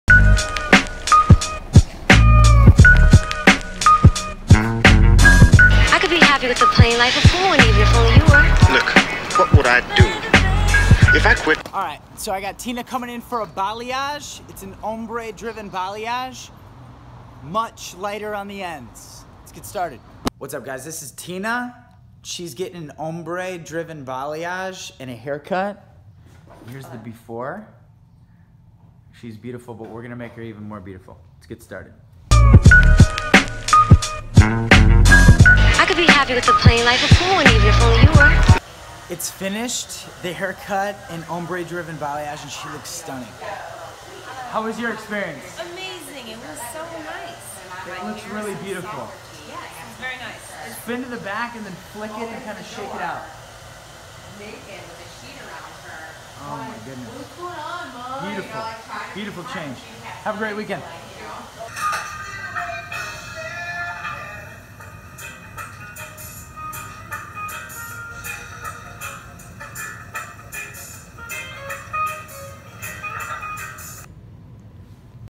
Tina's Ombre Balayage Transformation
Tina gets the Ombre Balayage of her dreams.
Follow Me on INTAGRAM: @eliotmunro,  @patricia_marie_salon